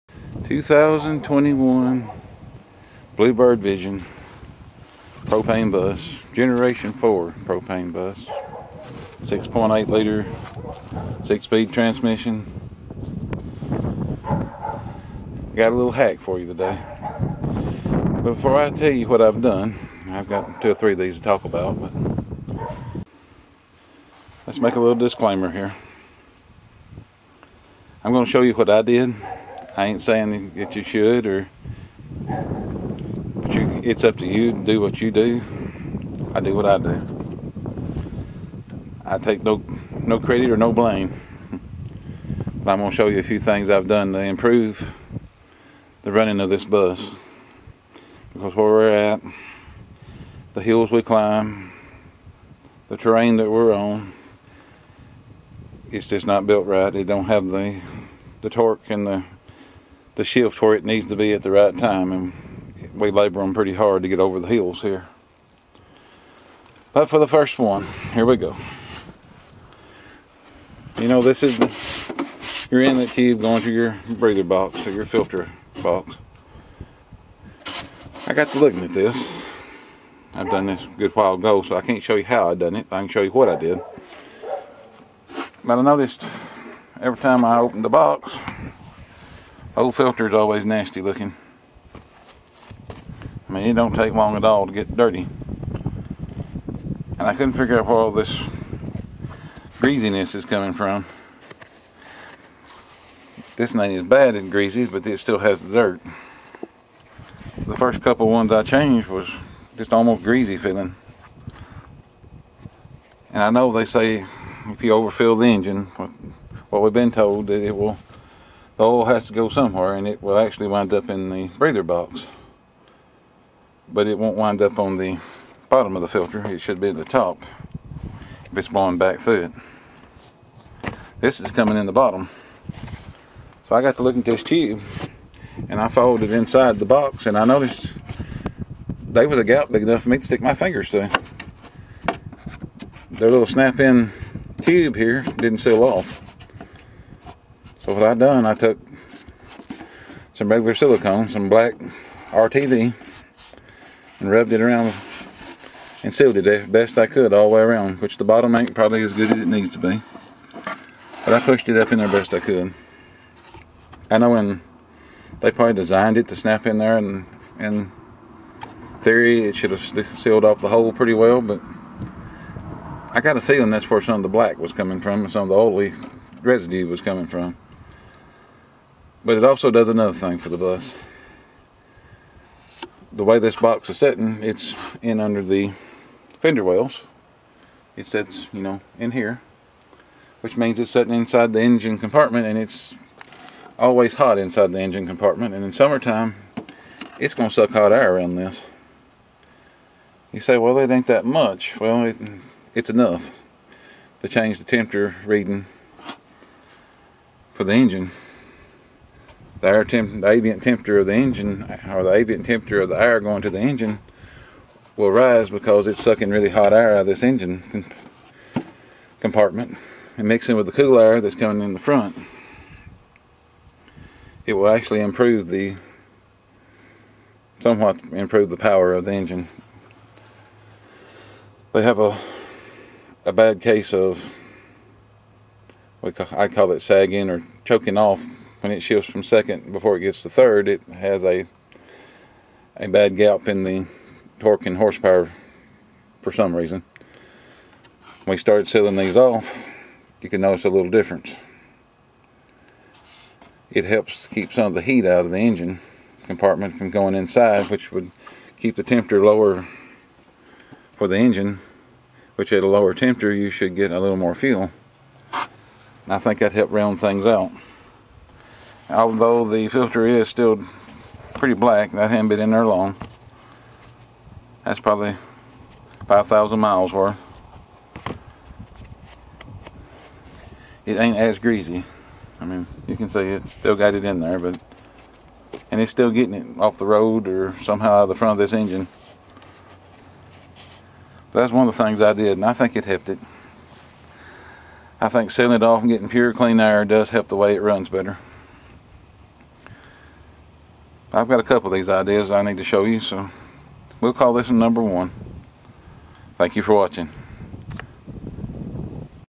2021 Bluebird Vision Propane Bus Simple Hack no. 1
Not exactly a hack but i think it helps to make the power more Consistent ... Does not effect the warranty!  Propane Engines Like Cool Air!